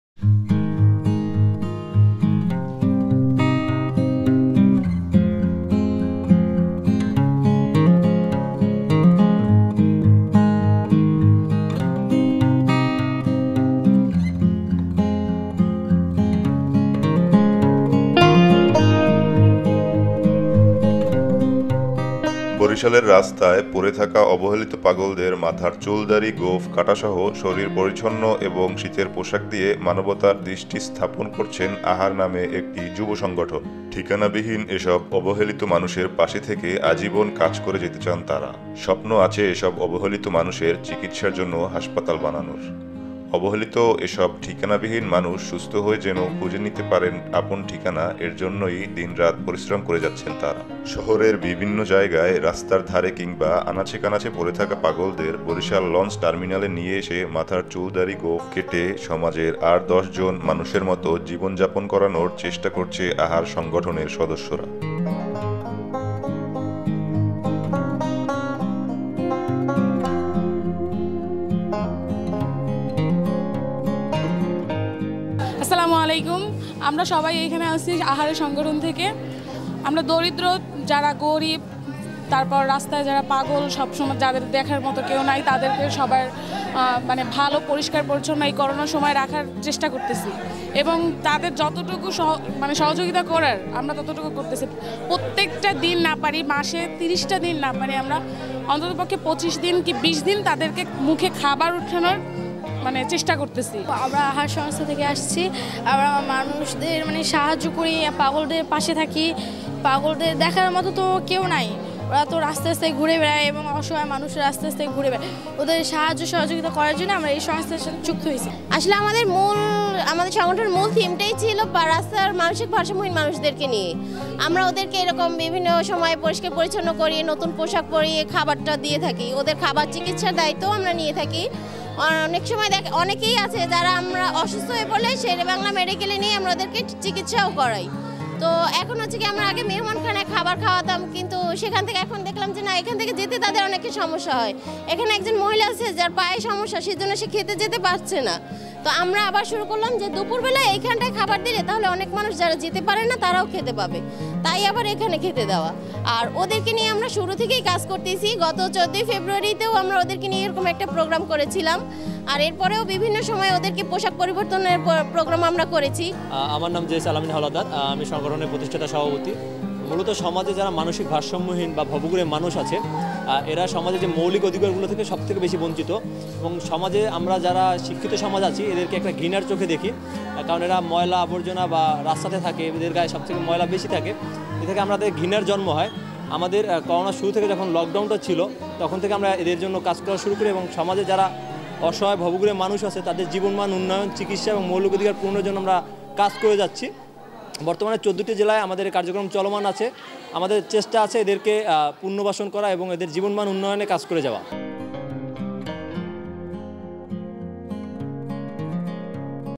বরিশালে আনাচে কানাচে পরে থাকা ভারসাম্যহীন মানুষদের সচেতন রূপে ফিরিয়ে দেয়ার ক্ষুদ্র প্রয়াশ
প্রতিষ্ঠাতাবরিশালে আনাচে কানাচে পরে থাকা ভারসাম্যহীন মানুষদের সচেতন রূপে ফিরিয়ে দেয়ার ক্ষুদ্র প্রয়াশ